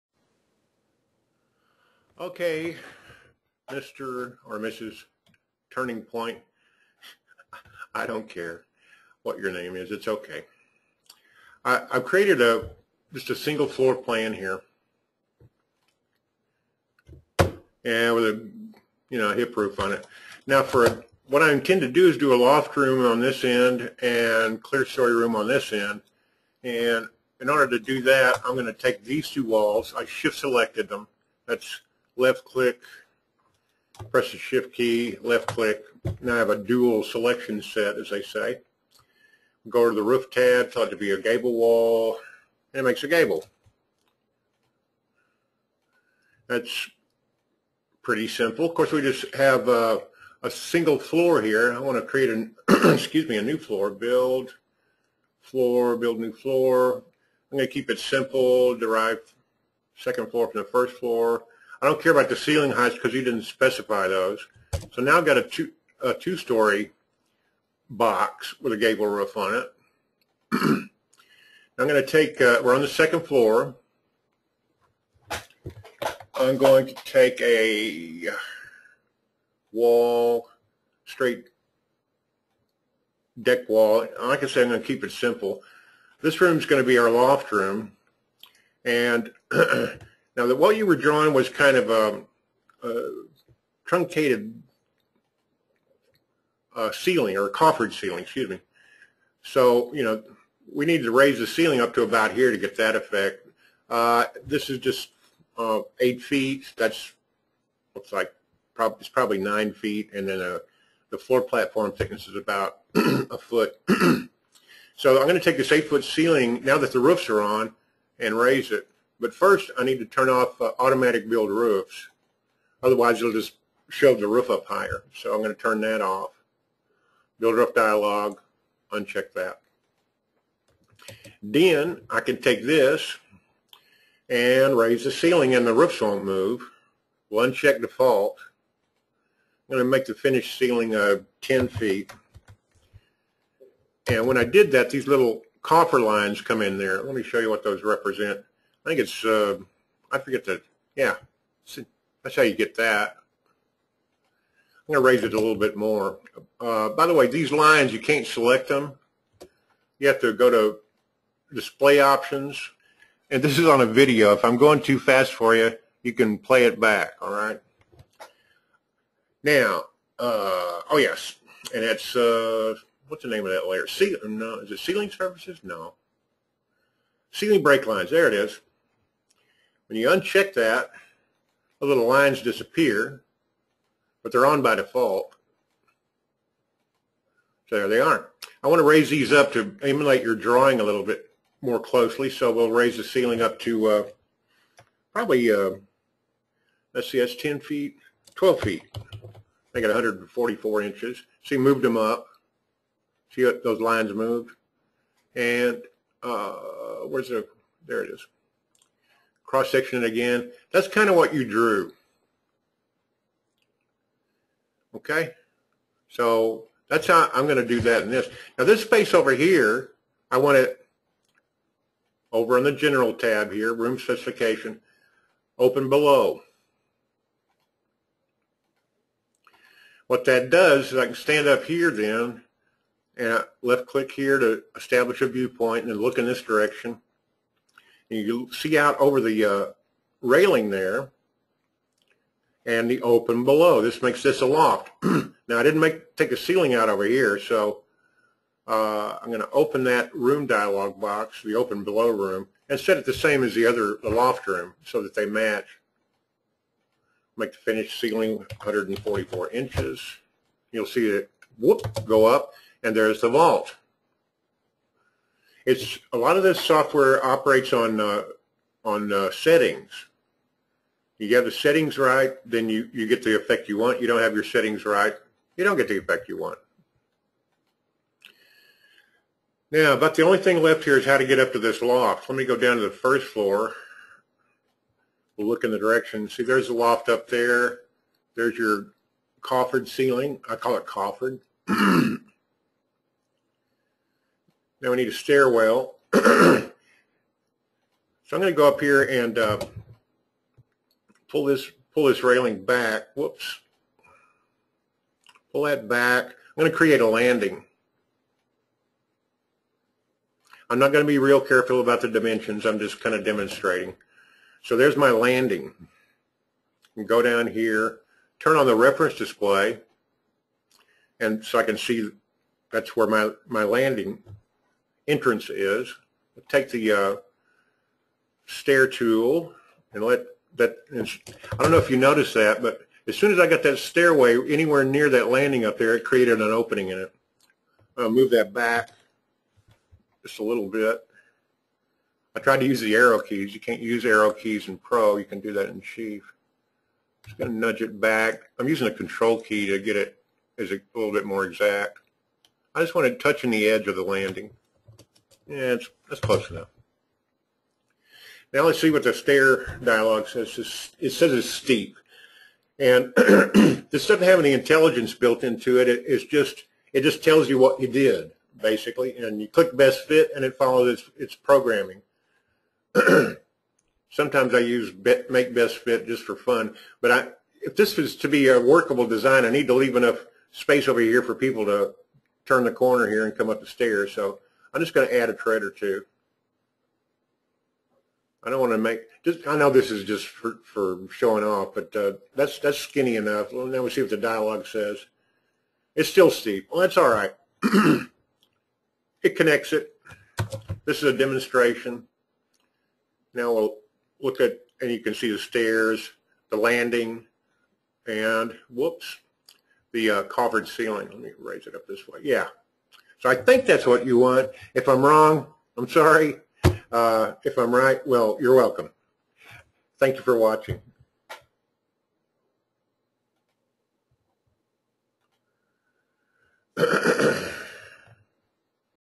Pro 2012 - 2- story, Clerestory with loft tutorial
How to do a simple two-story building with loft and coffered ceiling